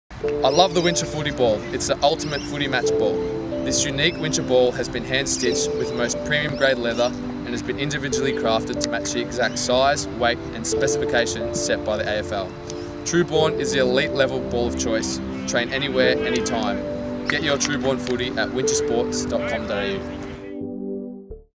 New Partnership | Winsher
Coburg has welcomed Winsher Sports as its official training ball supplier and partner until at least the end of 2023!

Winsher Sports will provide Coburg’s Men’s, Women’s and Junior footballers with the best quality training footballs that are conforming to all AFL football specifications, sizes and quality.